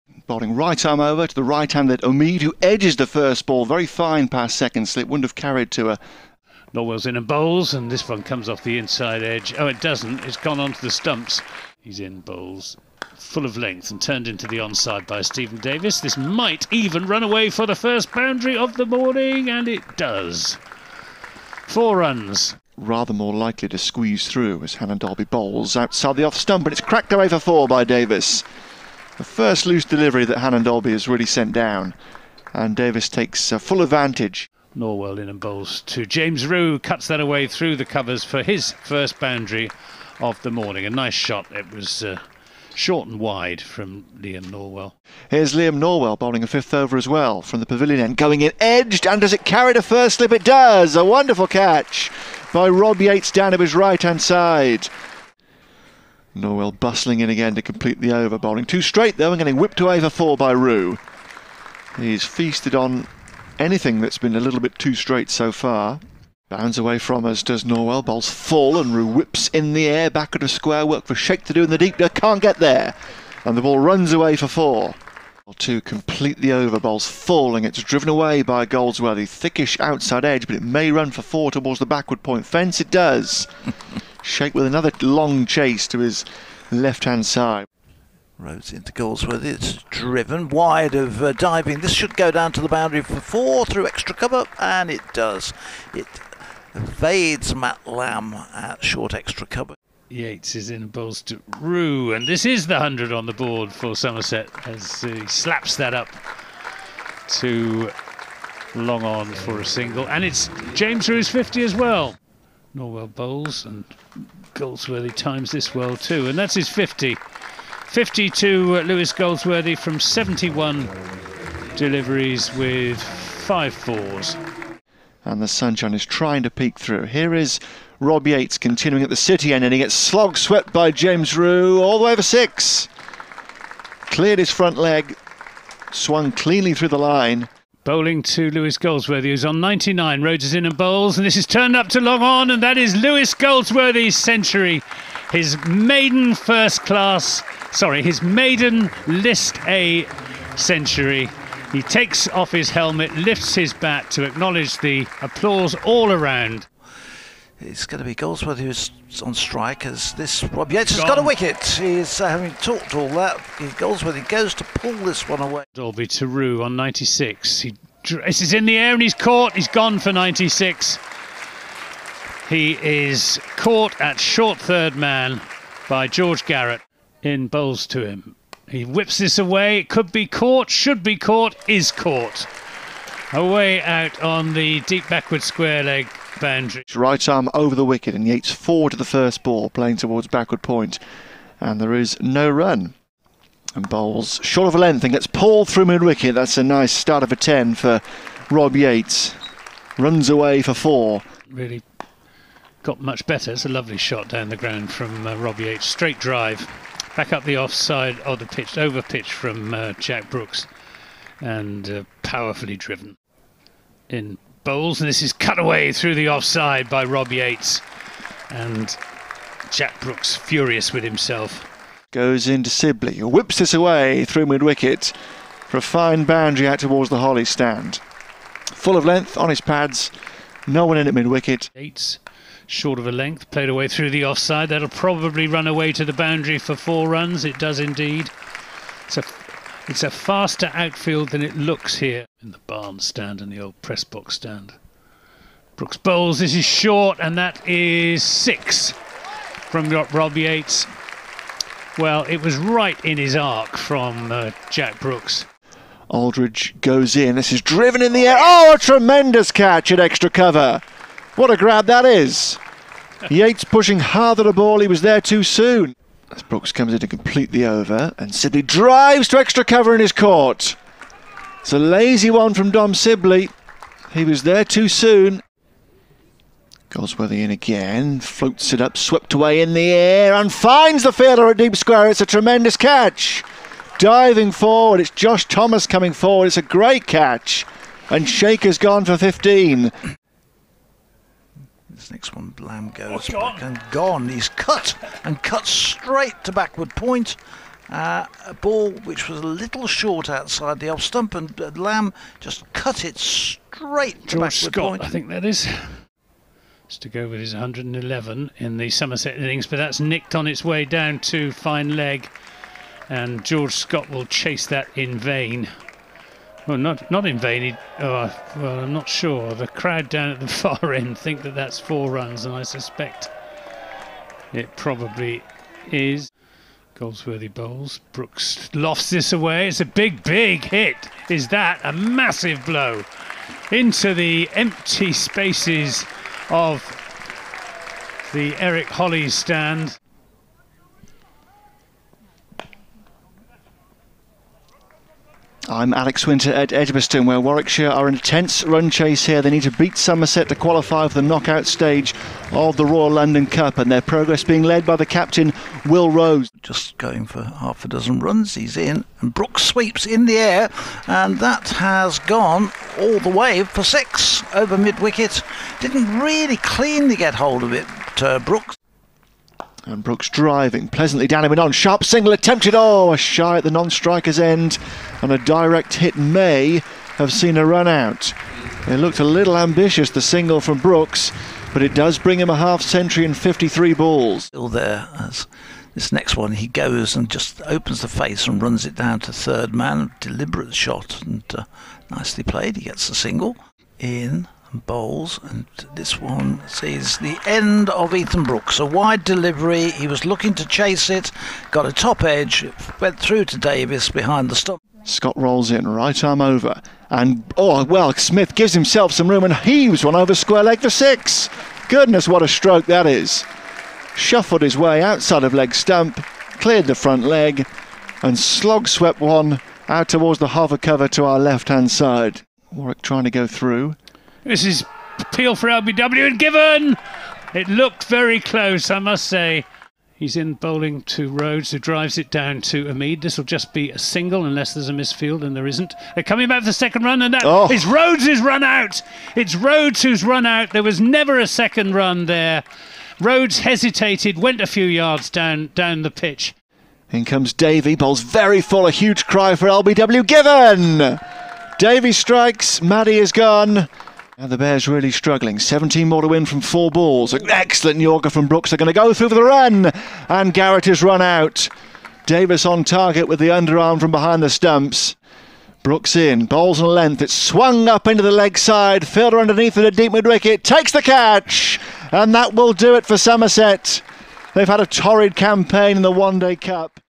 HIGHLIGHTS | Warwickshire v Somerset | Royal London Cup
Somerset collected their first Royal London Cup win at the eighth and last attempt and dragged Warwickshire out of the competition with them with a 14-run victory at Edgbaston.

Lewis Goldsworthy’s maiden List A century (111, 116 balls) and Jack Brooks’ excellent bowling (four for 38) underpinned a win which brought some belated light to a leaden campaign for the westcountrymen.